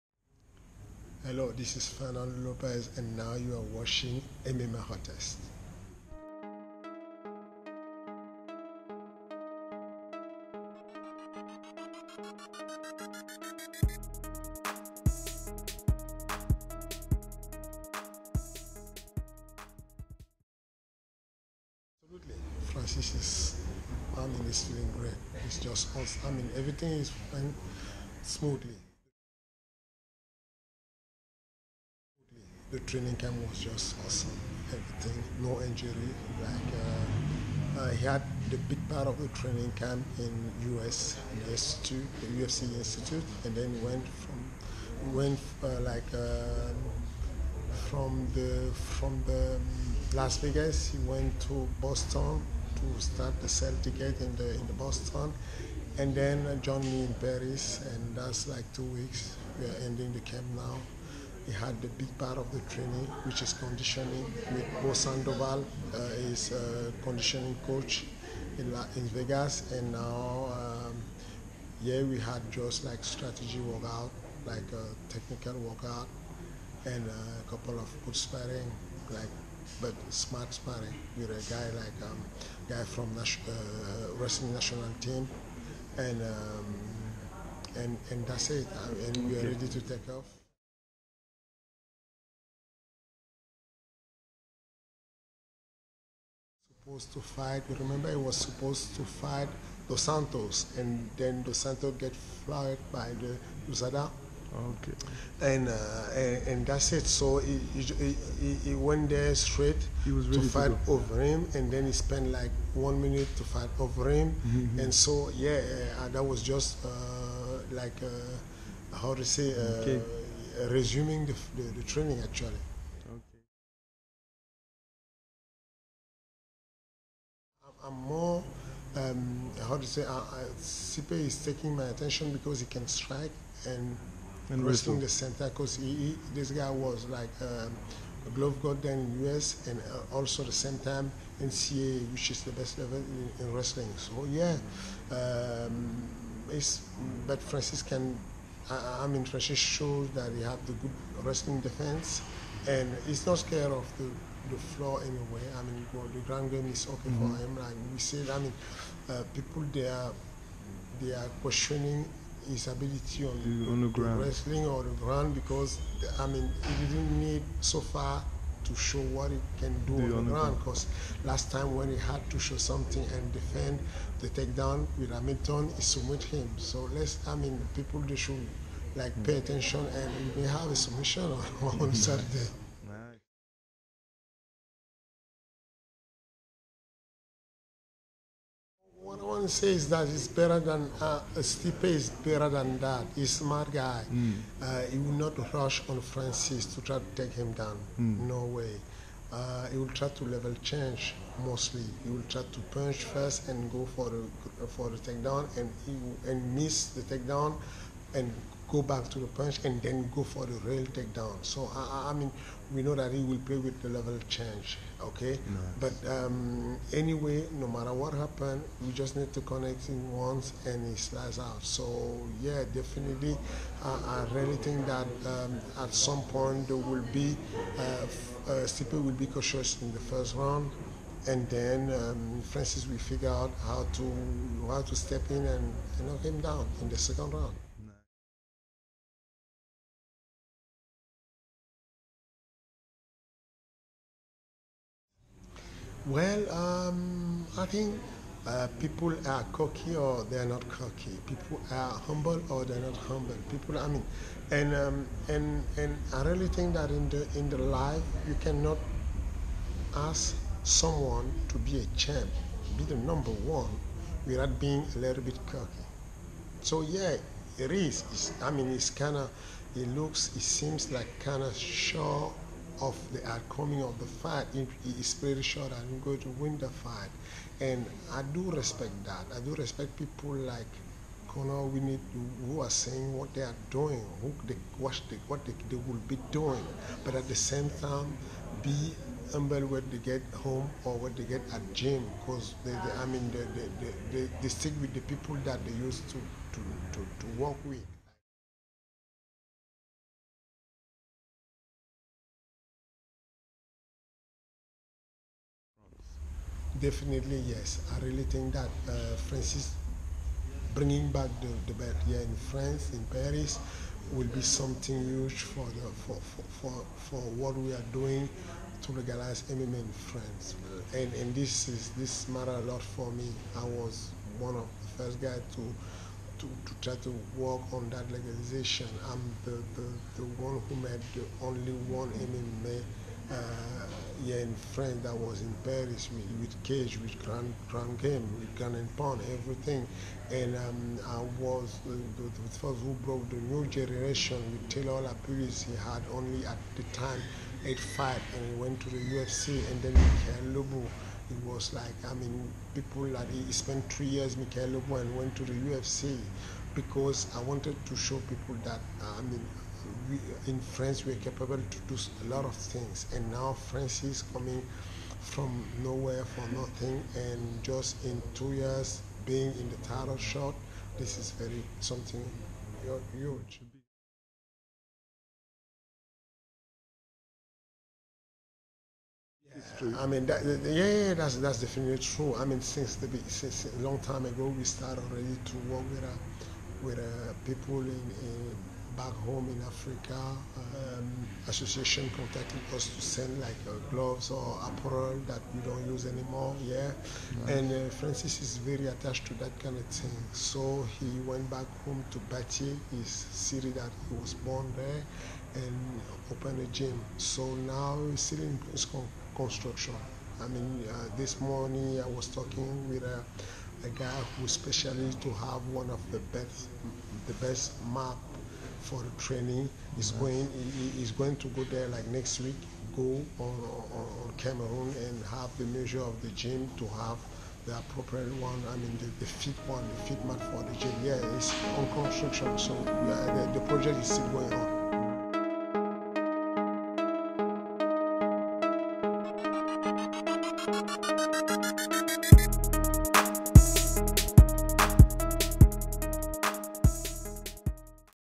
MMAH: Fernand Lopez Interview (Francis Nganou Head Coach)
This video is an interview with Francis Nganou Head Coach, Fernand Lopez, speaking on his upcoming Heavyweight Title fight vs. Stipe Miocic at UFC 220, Sunday, January 21, 2018, TD Garden, Boston, MA